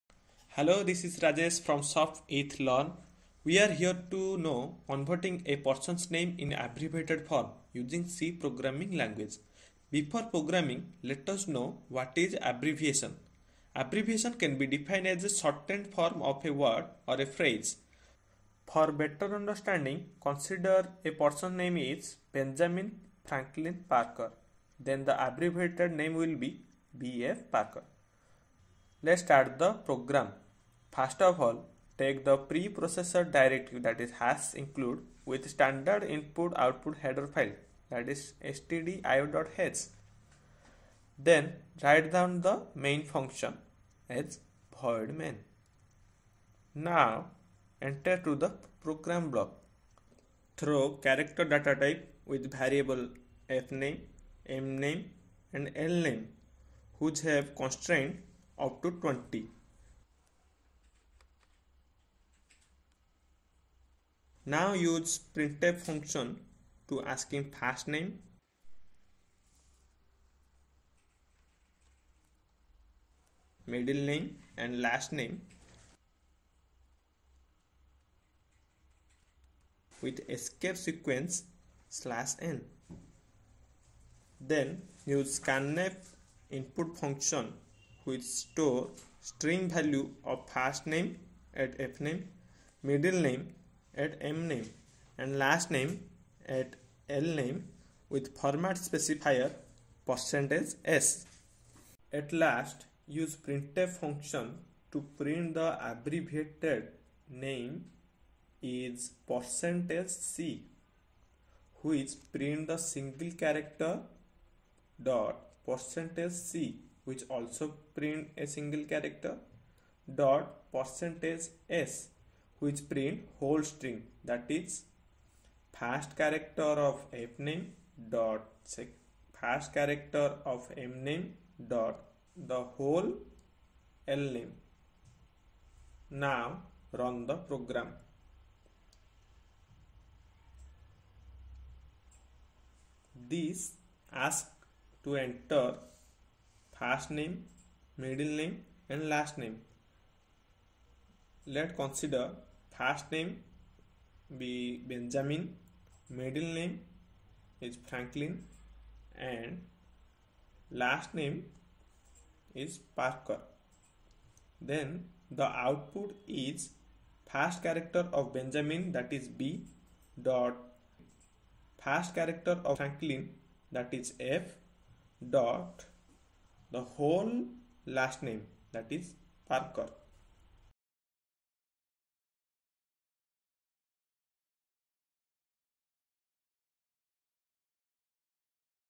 How to convert a person name in abbreviated form using c programming language || Softeath learn
void main()
       char fname[20], mname[20], lname[20];
       printf(“Enter first name, middle name and last name\n “);
       scanf(“%s %s %s”, fname, mname, lname);
       printf(“The abbreviated name is: %c. %c. %s”, fname[0], mname[0], lname“);